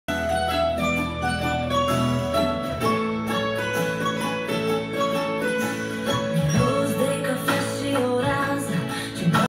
Inel de Logodna cu Diamante lucrat exclusiv manual
Inel de Logodna lucrat exclusiv manual in aur alb 18k. 2.64gr cu Diamant central : 0.31ct si Diamante anturaj : 0.12ct.
*50yearslifeguarantee
Code: WI00835Z0934